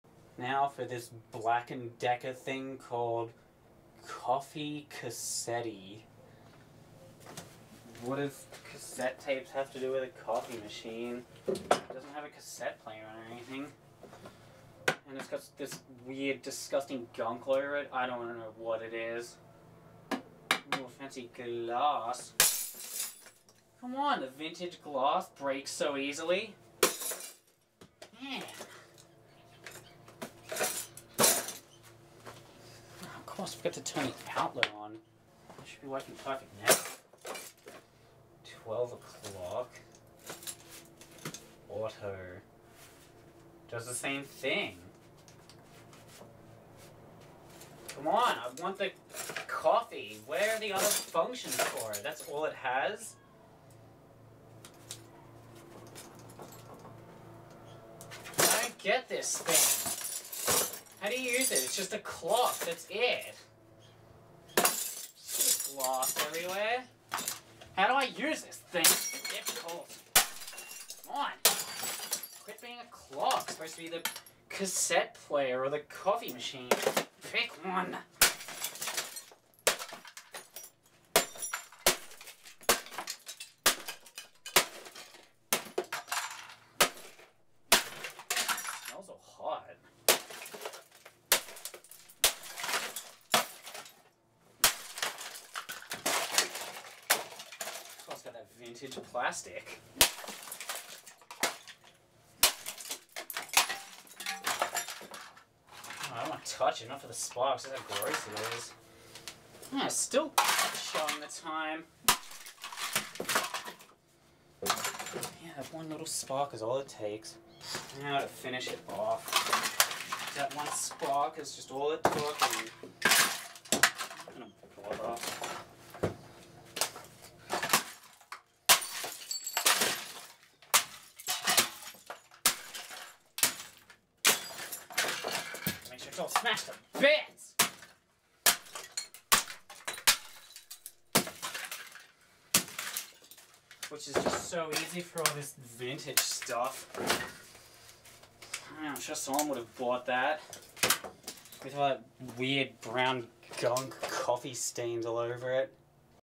Smash Vintage Black & Decker Coffee Cassette Machine
It didn't even have a cassette player, it lied!

Model: Black & Decker CDX4 coffee cassette machine.